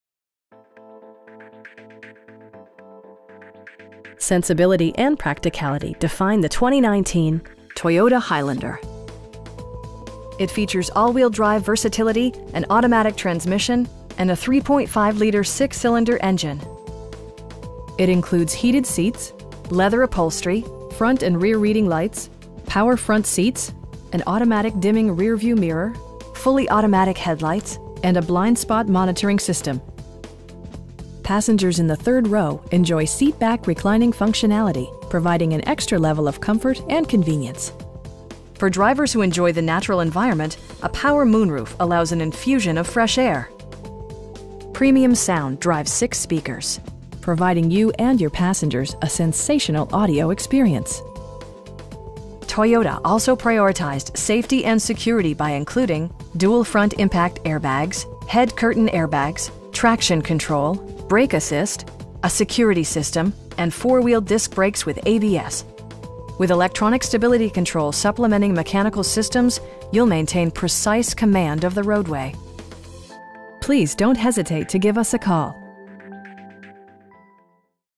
2019 Toyota Highlander XLE in Silver Spring, MD 20904
DARCARS Toyota of Silver Spring
12210 Cherry Hill Rd.

Sensibility and practicality define the 2019 Toyota Highlander. It features all-wheel drive versatility, an automatic transmission, and a 3.5 liter 6 cylinder engine. It includes heated seats, leather upholstery, front and rear reading lights, power front seats, an automatic dimming rear-view mirror, fully automatic headlights, power windows, and a blind spot monitoring system. Passengers in the third row enjoy seat back reclining functionality, providing an extra level of comfort and convenience. For drivers who enjoy the natural environment, a power moon roof allows an infusion of fresh air. Premium sound drives 6 speakers, providing you and your passengers a sensational audio experience. Toyota also prioritized safety and security by including: dual front impact airbags, head curtain airbags, traction control, brake assist, a security system, and 4 wheel disc brakes with ABS. With electronic stability control supplementing mechanical systems, you'll maintain precise command of the roadway. We pride ourselves in consistently exceeding our customer's expectations. Please don't hesitate to give us a call.